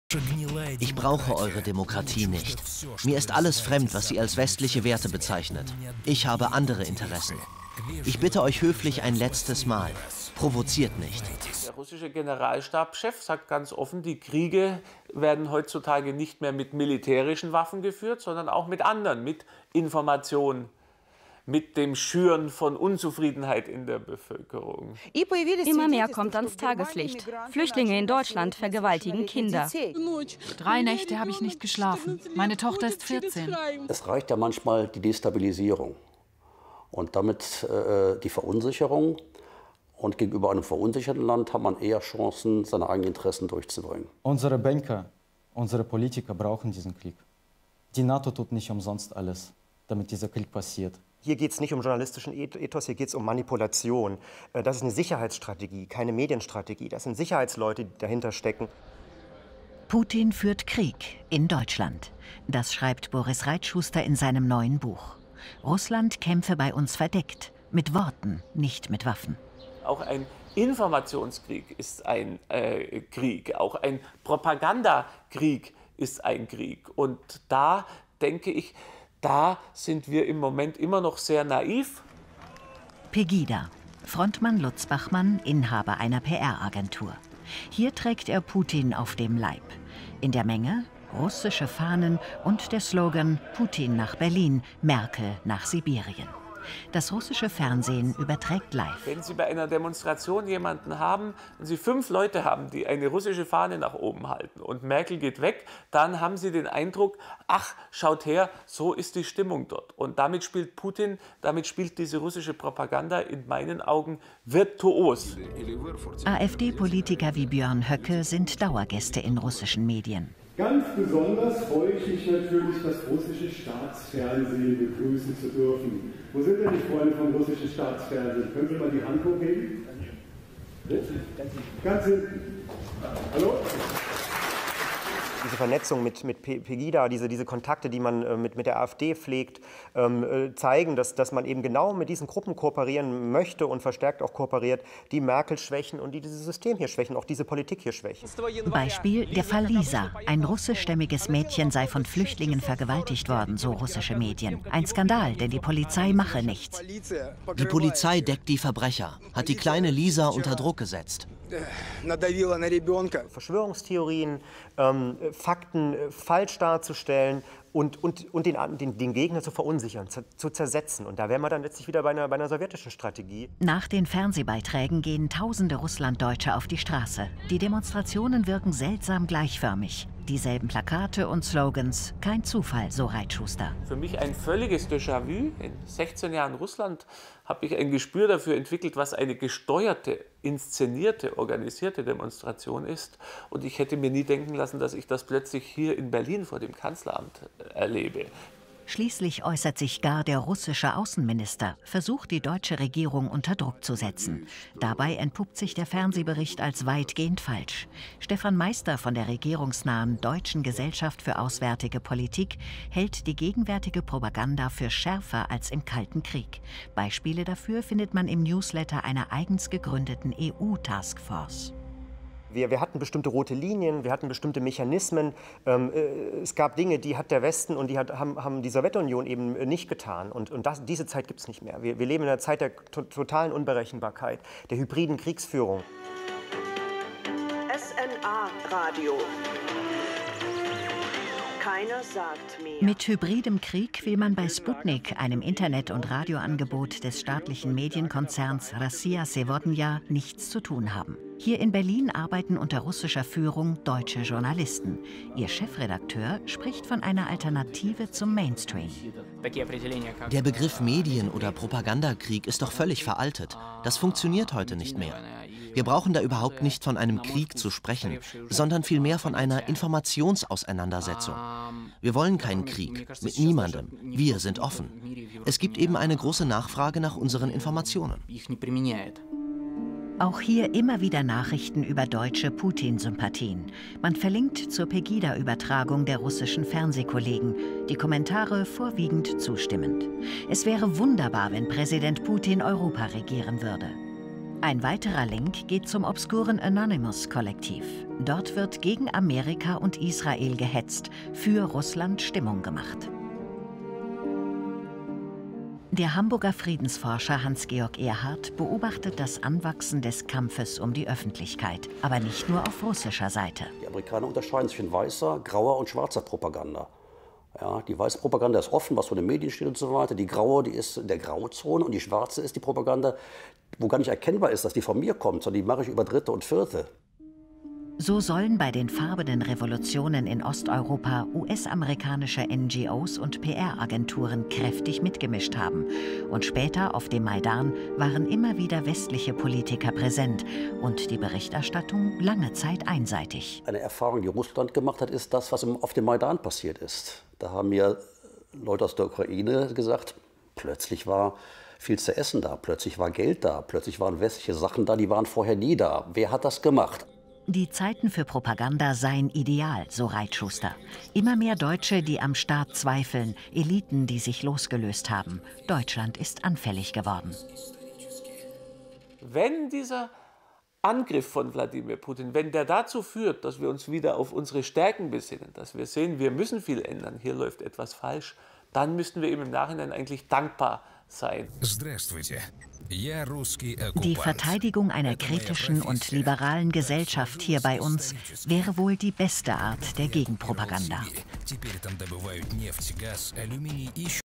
Agitprop, das bedeutet Agitation und Propaganda aus Russland, Teil 1
Deutscher Alltagswahnsinn: PEGIDA-Steuerung und AfD-Fremdbestimmung durch Russland, PUTIN-Wahlkampf..
Steuerung, Zensur und russische Zersetzungsmethoden gegen die freiheitlich-demokratische Grundordnung und unsere Regierung!